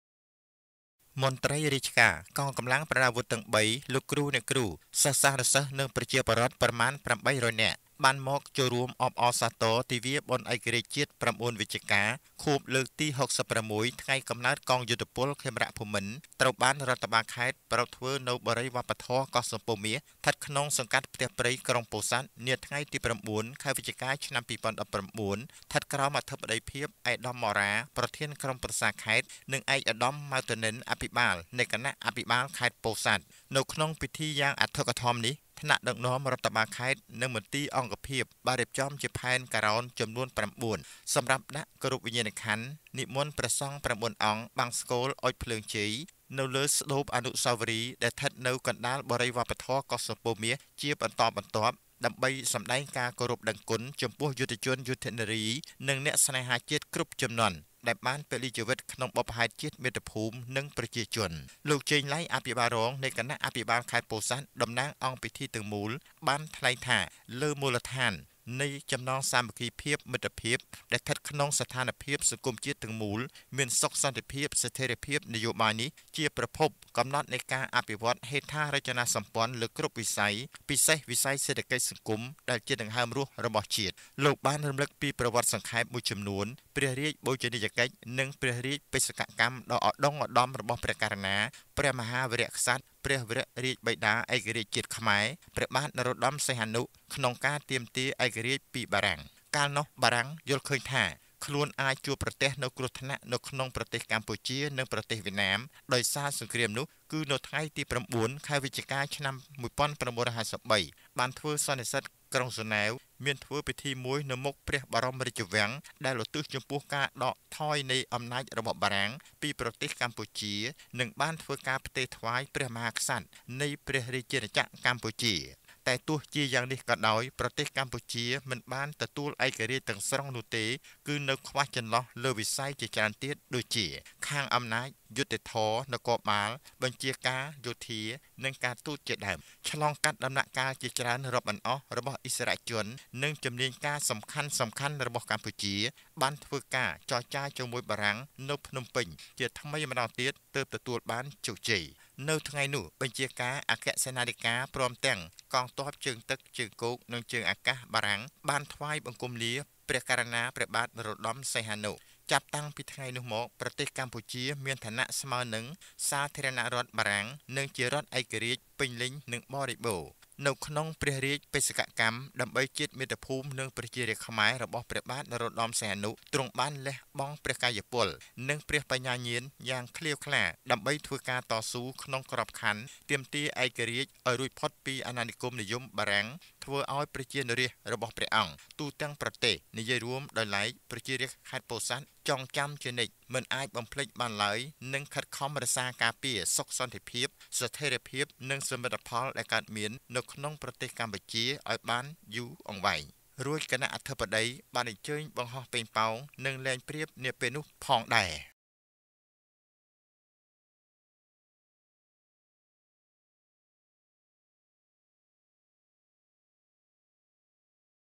បុណ្យឯករាជ្យជាតិ៩វិច្ឆិកាខួបលើកទី៦៦ និងកំណើតកងយោធពលខេមរភូមិន្ទនៅខេត្តពោធិ៍សាត់
មន្ត្រីរាជការ កងកម្លាំងប្រដាប់អាវុធទាំង៣ លោកគ្រូ អ្នកគ្រូសិស្សានុសិស្សនិងប្រជាពលរដ្ឋប្រមាន៨០០នាក់ បានមក ចូលរួមអបអរសាទរទិវាបុណ្យឯករាជ្យជាតិ៩វិច្ឆិកាខួបលើកទី៦៦ថ្ងៃកំណើតកងយោធពលខេមរភូមិន្ទ ត្រូវបានរដ្ឋបាលខេត្តប្រា រព្ធធ្វើនៅបុរីវប្បធម៌កោះសំពៅមាស ស្ថិតក្នុងសង្កាត់ផ្ទះព្រៃ ក្រុងពោធិ៍សាត់នាថ្ងៃទី០៩ខែវិច្ឆិកា ឆ្នាំ២០១៩ ស្ថិតក្រោម អធិបតីភាពឯកឧត្តម ម៉ក់ រ៉ា ប្រធានក្រុមប្រឹក្សាខេត្ត និងឯកឧត្តម ម៉ៅ ធនិន អភិបាលនៃគណៈអភិបាលខេត្តពោធិ៍សាត់។
 នៅក្នុងពិធីយ៉ាងអធិកអធមនេះថ្នាក់ដឹកនាំរដ្ឋបាលខេត្តនិងមន្ទីរ អង្គភាព បានរៀបចំជាផែនការ៉ោន ចំនួន៩សម្រាប់ដាក់ គោរព វិញ្ញាណក្ខន្ធនិមន្តព្រះសង្ឃ៩អង្គ បង្សុកូល អុជភ្លើងជ័យ នៅលើស្តូប អនុស្សាវរីយ៍ដែលស្ថិតនៅ កណ្តាលបុរីវប្បធម៌ កោះសំពៅមាសជាបន្តបន្ទាប់ ដើម្បីសម្តែងការគោរពដឹងគុណចំពោះយុទ្ធជន យុទ្ធនារីនិងអ្នកស្នេហាជាតិគ្រប់ជំនាន់ ដែលបាន ពលីជីវិតក្នុងបុព្វហេតុជាតិមាតុភូមិនិងប្រជាជន។
 លោក ចេង ឡៃ អភិបាលរងនៃគណៈអភិបាលខេត្តពោធិ៍សាត់តំណាងអង្គពិធីទាំងមូល បានថ្លែងថាលើមូលដ្ឋាន នៃចំណងសាមគ្គីភាព មិត្តភាព ដែលស្ថិតក្នុងស្ថានភាពសង្គមជាតិទាំងមូល មានសុខសន្តិភាព ស្ថេរភាព នយោបាយនេះ ជា ប្រភពកំណត់នៃការនិងការអភិវឌ្ឍន៍ហេដ្ឋារចនាសម្ព័ន្ធលើគ្រប់វិស័យពិសេសវិស័យសេដ្ឋកិច្ចសង្គមដែលជាដង្ហើមរស់របស់ជាតិ។ លោកបានរំលឹកពីប្រវត្តិសង្ខេបមួយចំនួនព្រះរាជបូជនីយកិច្ចនិងព្រះរាជបេសកកម្មដ៏ឧត្តុង្គឧត្តមរបស់ព្រះករុណា ព្រះមហា វីរក្សត្រ ព្រះវរាជបិតាឯករាជជាតិខ្មែរព្រះបាទនរោត្តម សីហនុ ក្នុងការទាមទាឯករាជ្យពីបារាំង។ កាលនោះបារាំងយល់ឃើញថា ខ្លួនអាចជួបប្រទះនូវគ្រោះថ្នាក់នៅក្នុងប្រទេសកម្ពុជានិងនៅវៀតណាមដោយសារសង្គ្រាមនោះគឺនៅថ្ងៃទី៩វិច្ឆិកាឆ្នាំ១៩៥៣ បាន ធ្វើសន្និសីទ្ធក្រុងហ្សីណែវមានធ្វើពិធីមួយនៅមុខព្រះបរមរាជវាំងដែលឧទ្ទិសចំពោះការដកថយនៃអំណាចរបស់បារាំង ពីប្រទេស កម្ពុជានិងបានធ្វើការផ្ទេរថ្វាយព្រះមហាក្សត្រនៃព្រះរាជាណាចក្រកម្ពុជា។តែទោះជាយ៉ាងនេះក៏ដោយប្រទេសកម្ពុជាមិនបានទទួលឯករាជទាំងស្រុងនោះទេគឺនៅខ្វះចន្លោះលើវិស័យជាច្រើនទៀតដូចជា ខាងអំណាច យុត្តិធម៌ នគរបាល បញ្ជាការ យោធា និងការទូតជាដើម ។ឆ្លងកាត់ដំណាក់កាលជាច្រើនរាប់មិនអស់របស់ឥស្សរជននិងជំនាញការសំខានៗរបស់កម្ពុជា បានធ្វើការចរចារជាមួយបារាំងនៅភ្នំពេញជាថ្មីម្តងទៀតទើបទទួលបានជោគជ័យ។នៅថ្ងៃនោះបញ្ជាកាអគ្គសេនាធិការព្រមទាំងកង ទ័ព ជើងទឹក ជើងគោក និងជើងអាកាសបារាំងបានថ្វាយបង្គំលា ព្រះករុណាព្រះបាទនរោត្តម សីហនុ។ ចាប់តាំងពីថ្ងៃនោះ មកប្រទេសកម្ពុជាមានឋានៈស្មើនឹងសាធារណរដ្ឋបារាំងនិងជារដ្ឋឯករាជ្យពេញលេញនិងបរិបូរណ៍ ។ នៅក្នុងព្រះរាជបេសកកម្ម ដើម្បីជាតិមាតុភូមិ និងប្រជារាស្ត្រ ខ្មែររបស់ព្រះបាទនរោត្តមសីហនុ ទ្រង់បានលៈបង់ព្រះកាយពល និងព្រះបញ្ញាញាណយ៉ាង ក្លៀវក្លាដើម្បីធ្វើការតស៊ូ ក្នុងក្របខណ្ឌទាមទាឯករាជ្យឱ្យរួចផុតពីអាណានិគមនិយមបារាំង ធ្វើឱ្យប្រជានុរាស្ត្ររបស់ព្រះអង្គទូទាំង ប្រទេសនិយាយរួមដោយឡែកប្រជារាស្ត្រខេត្តពោធិ៍សាត់ចងចាំជានិច្ចមិនអាចបំភ្លេចបានឡើយនិងខិតខំរក្សាការពារសុខសន្តិ ភាព ស្ថេរភាពនិងសមិទ្ធិផលដែលកើតមាននៅក្នុងប្រទេសកម្ពុជាឱ្យបានបានយូរអង្វែង រួចគណៈអធិបតីបានអញ្ជើញ បង្ហោះប៉េងប៉ោងនិងលេងព្រាបនាពេលនោះផងដែរ ៕ ស្ងួន ឈឿន ពោធិ៍សាត់